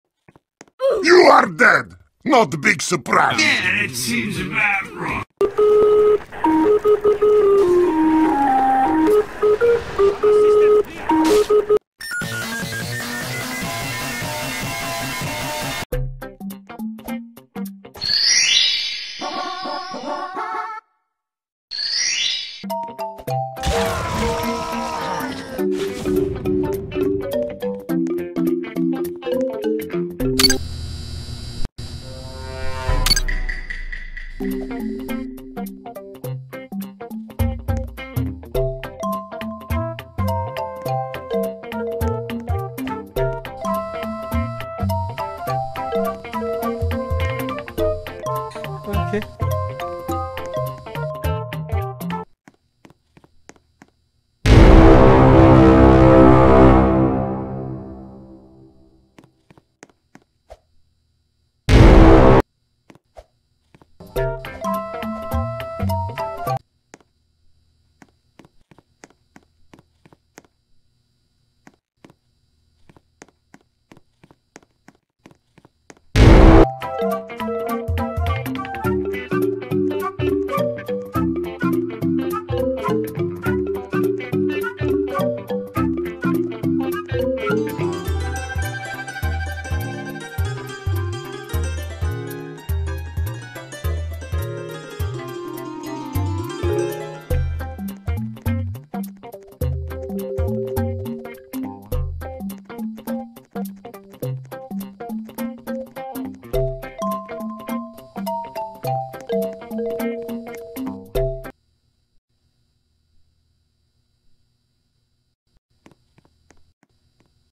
So i tried out my friend's roblox game-
"I have a special delivery, just for you!"
Hello there, it's me, ImFeelingNotOk, and today we are back with a new animation/video...

Often used models:
Scott Cawthon's/Steel Wool Studio's FNaF models (ported by teTtris)
Some Random models by Leymi and Sagiro (?)
Bendy models by Joey Drew Studios
Pokemon models by GameFreak